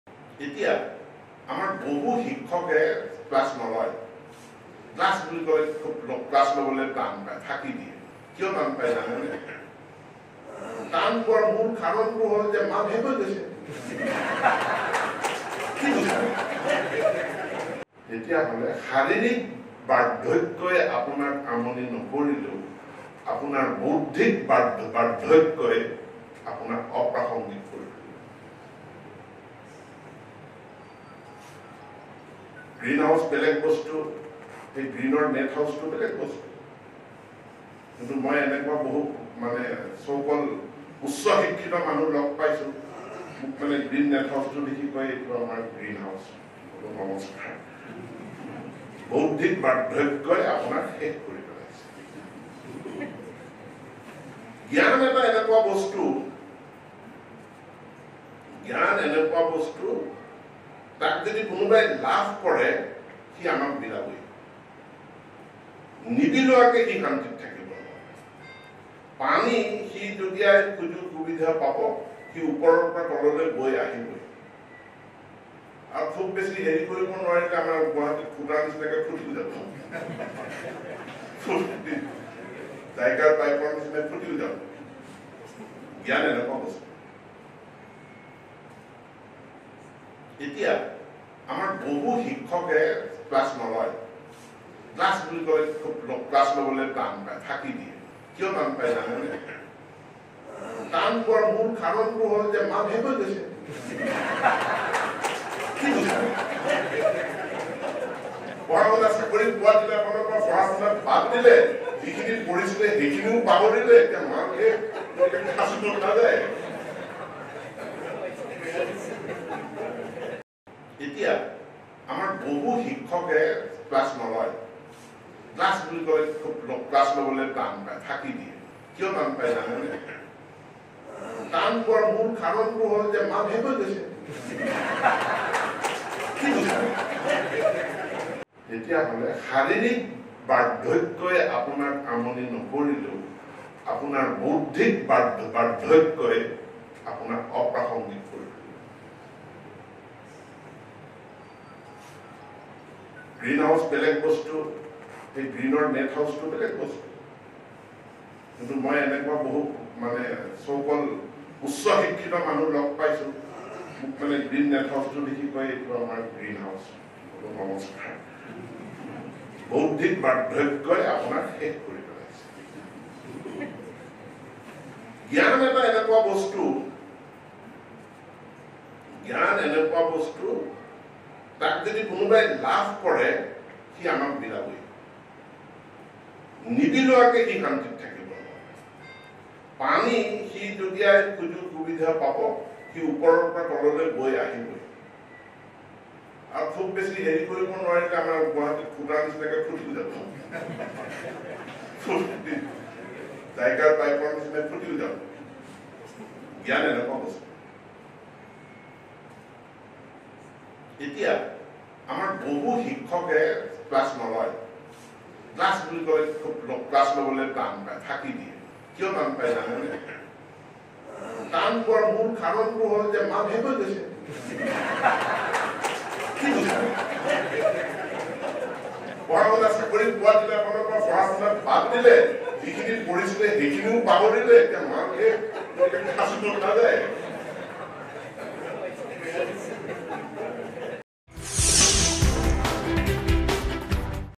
''আজিকালি একাংশ শিক্ষকৰ মগজুত মাল পানী নোহোৱা হৈছে।
''আজিকালি একাংশ শিক্ষকৰ মগজুত মাল পানী নোহোৱা হৈছে। সেইসকল শিক্ষকে ক্লাছ নকৰে। কাৰণ সেইচাম শিক্ষকে পঢ়া শুনা নকৰে,অধ্যয়ন নকৰে''। শিক্ষাগুৰুক চৰম অপমান শিক্ষামন্ত্ৰী ৰণোজ পেগুৰ